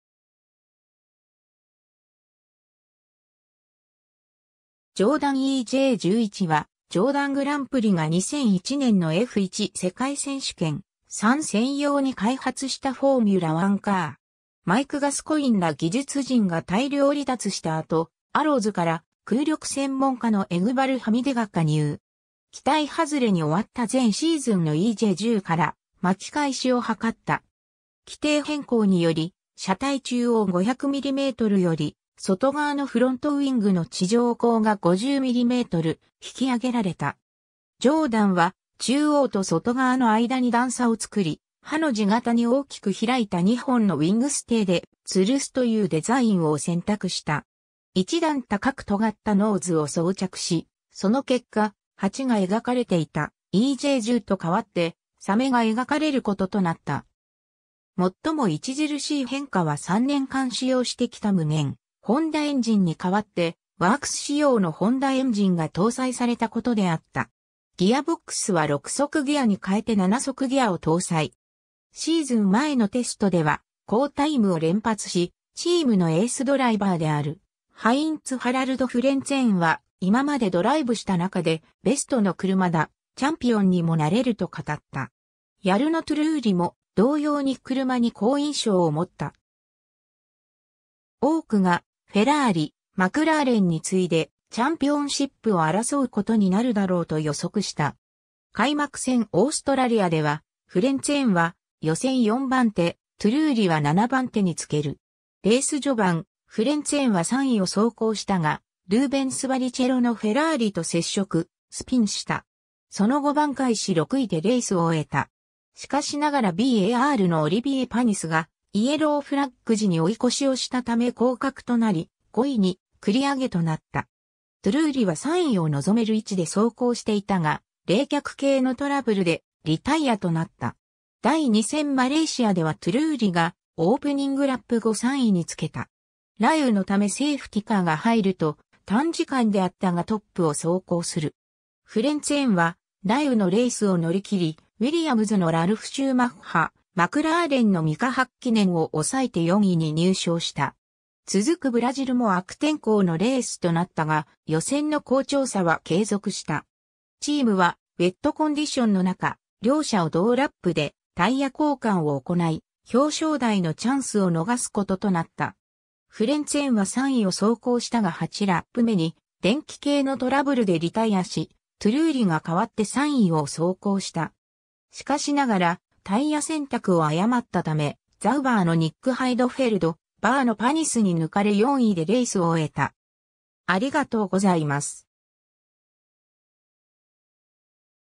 ジョーダン・EJ11
ジョーダン・グランプリのF1マシン
ジョーダン・EJ11 (Jordan EJ11) は、ジョーダン・グランプリが2001年のF1世界選手権参戦用に開発したフォーミュラ1カー。
 マイク・ガスコインら技術陣が大量離脱した後、アロウズから空力専門家のエグバル・ハミディが加入。
期待外れに終わった前シーズンのEJ10から巻き返しを図った。
 規定変更により、車体中央500mmより外側のフロントウィングの地上高が50mm引き上げられた。
ジョーダンは中央と外側の間に段差をつくり、ハの字型に大きく開いた2本のウィングステーで吊るすというデザインを選択した。
一段高く尖ったノーズを装着し、その結果、蜂が描かれていたEJ10と変わって鮫（シャークマウス）が描かれることとなった）。
 最も著しい変化は3年間使用してきた無限ホンダエンジンに代わってワークス仕様のホンダエンジンが搭載されたことであった。
ギアボックスは6速ギアに変えて7速ギアを搭載。
 シーズン前のテストでは好タイムを連発し、チームのエースドライバーであるハインツ＝ハラルド・フレンツェンは今までドライブした中でベストの車だ、チャンピオンにもなれると語った。
ヤルノ・トゥルーリも同様に車に好印象を持った。
多くがフェラーリ、マクラーレンに次いでチャンピオンシップを争うことになるだろうと予測した。
 開幕戦オーストラリアではフレンツェンは予選4番手、トゥルーリは7番手につける。
レース序盤、フレンツェンは3位を走行したが、ルーベンス・バリチェロのフェラーリと接触、スピンした。
その後挽回し6位でレースを終えた。
しかしながらB・A・Rのオリビエ・パニスがイエローフラッグ時に追い越しをしたため降格となり、5位に繰り上げとなった。
トゥルーリは3位を望める位置で走行していたが、冷却系のトラブルでリタイアとなった。
 第2戦マレーシアではトゥルーリがオープニングラップ後3位につけた。
雷雨のためセーフティカーが入ると、短時間であったがトップを走行する。
フレンツェンは雷雨のレースを乗り切り、ウィリアムズのラルフ・シューマッハ、マクラーレンのミカ・ハッキネンを押さえて4位に入賞した。
 続くブラジルも悪天候のレースとなったが、予選の好調さは継続した。
チームはウェットコンディションの中、両車を同ラップでタイヤ交換を行い、表彰台のチャンスを逃すこととなった。
フレンツェンは3位を走行したが8ラップ目に電気系のトラブルでリタイアし、トゥルーリが代わって3位を走行した。
しかしながらタイヤ選択を誤ったため、ザウバーのニック・ハイドフェルド、BARのパニスに抜かれ4位でレースを終えた。